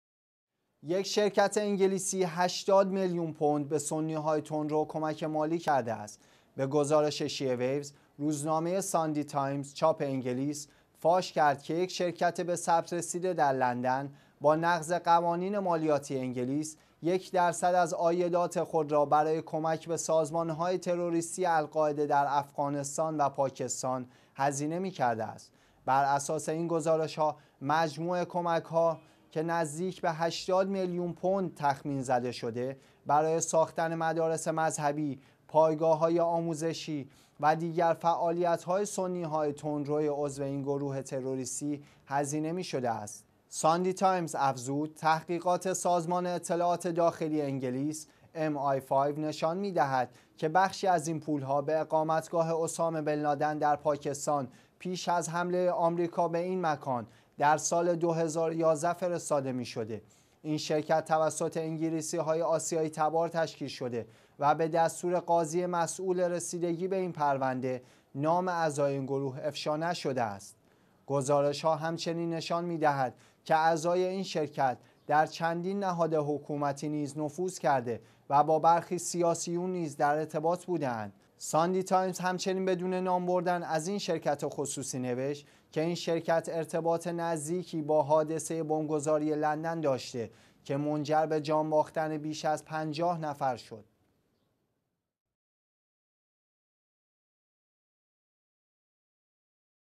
افشای کمک ده ها میلیون پوندی یک شرکت انگلیسی به سنی های تندرو
یک شرکت انگلیسی،  80 میلیون پوند به سنی های تندرو کمک مالی کرده است.
به گزارش شیعه ویوز، روزنامه «ساندی تایمز» چاپ انگلیس، فاش کرد که یک شرکت به ثبت رسیده در لندن، با نقض قوانین مالیاتی انگلیس، یک درصد از عایدات خود را برای کمک به سازمان تروریستی القاعده در افغانستان و پاکستان هزینه می‌کرده است.
براساس این گزارش، مجموع کمک‌ها که نزدیک به 80 میلیون پوند تخمین زده شده، برای ساختن مدارس مذهبی، پایگاه‌های آموزشی و دیگر فعالیت‌های سنی های تندرویِ عضو این گروه تروریستی، هزینه می‌شده است.
ساندی تایمز افزود: تحقیقات سازمان اطلاعات داخلی انگلیس (ام آی 5) نشان می‌دهد که بخشی از این پول‌ها به اقامتگاه «اسامه بن لادن» در پاکستان (پیش از حمله آمریکا به این مکان در سال 2011)  فرستاده می‌شده.
این شرکت توسط انگلیسی های آسیایی تبار تشکیل شده،  و به دستور قاضی مسئول رسیدگی به این پرونده،  نام اعضای این گروه افشا نشده است.
گزارش‌ها همچنین نشان می‌دهد که اعضای این شرکت در چندین نهاد حکومتی نیز نفوذ کرده و با برخی سیاسیون نیز در ارتباط بوده اند.
ساندی تایمز همچنین بدون نام بردن از این شرکت خصوصی،  نوشت که این شرکت  ارتباط نزدیکی با حادثه ی بمب‌گذاری لندن داشته که منجر به جان باختن بیش از 50 نفر شد.